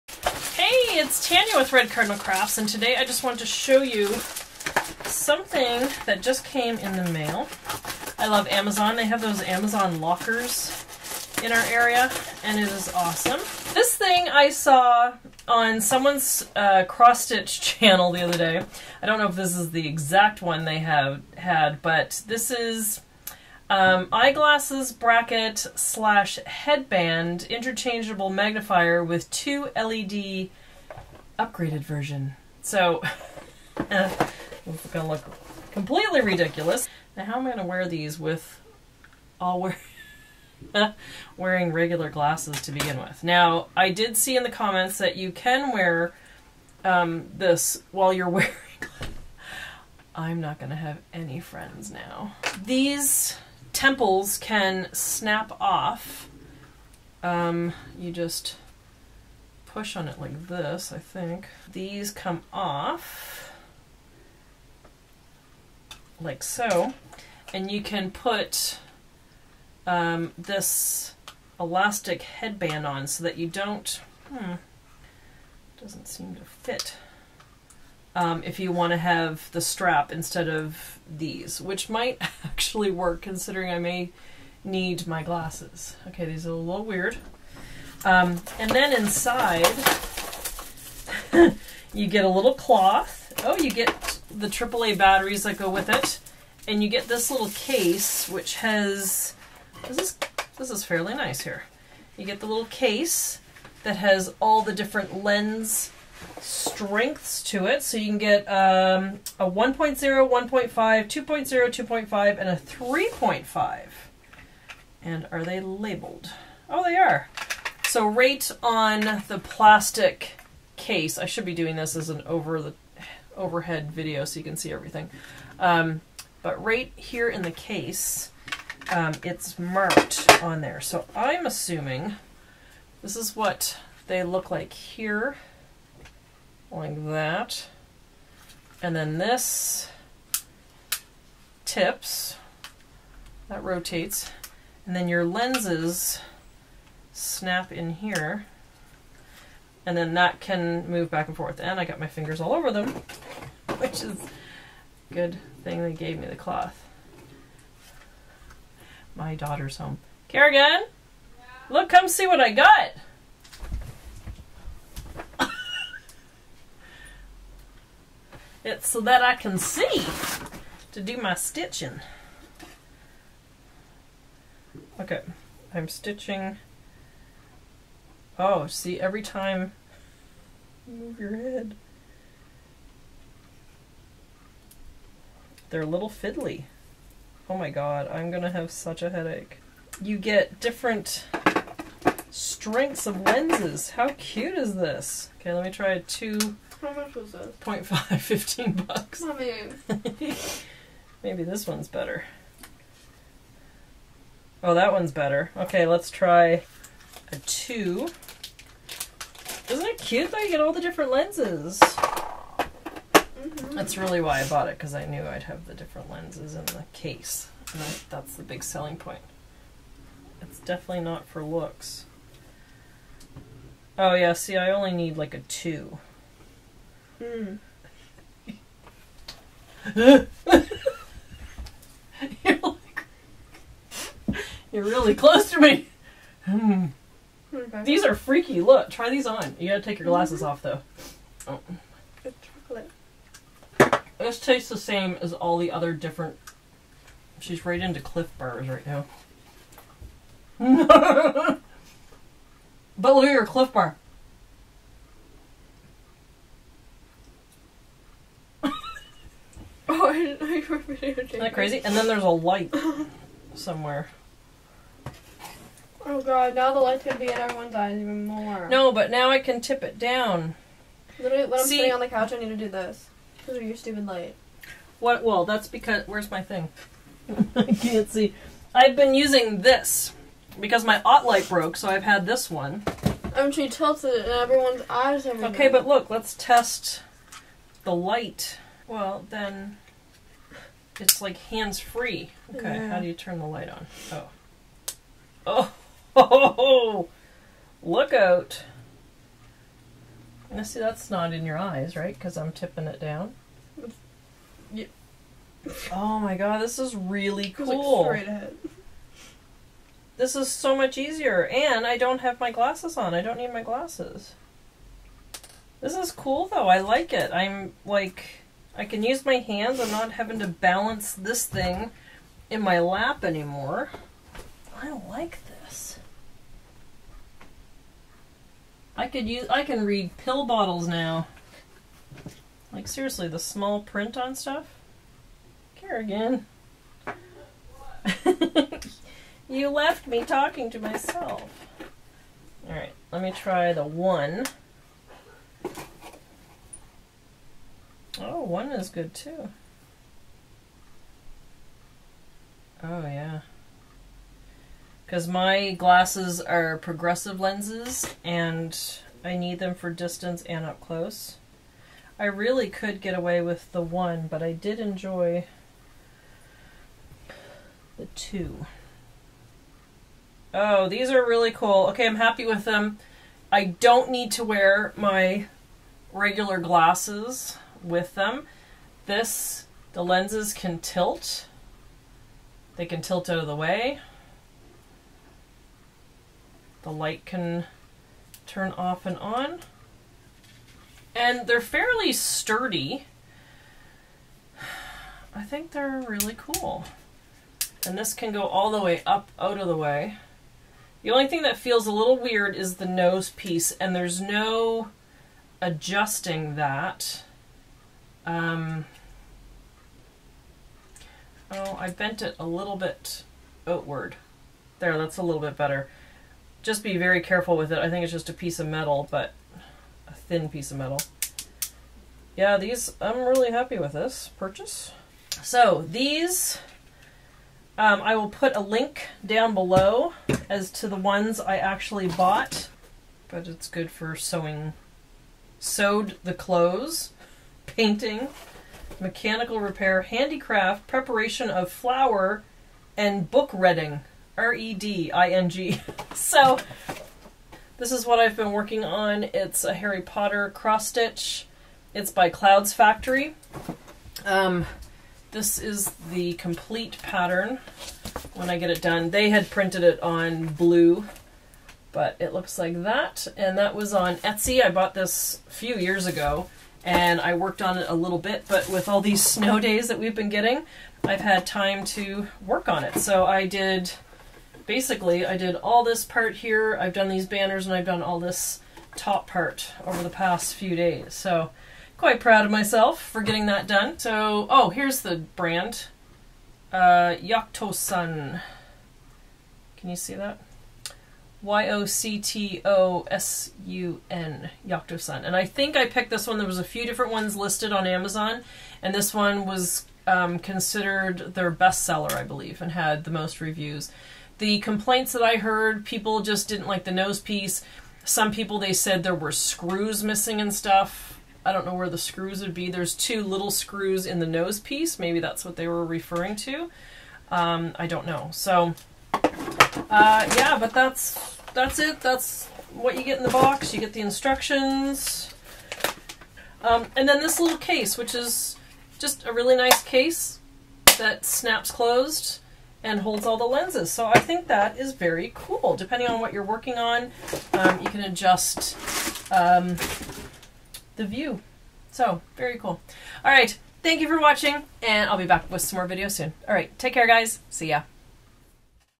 HANDS FREE MAGNIFIER WITH LIGHT Video #196
Hi Guys, I thought I would show you the new magnifier I purchased. Take care.

***There are companies out there stealing my images from this video and using it in their own advertisements. I have not authorized this use. Thank you so much

Head mount magnifier:

My children's book, Squirrel's Peanut Allergy: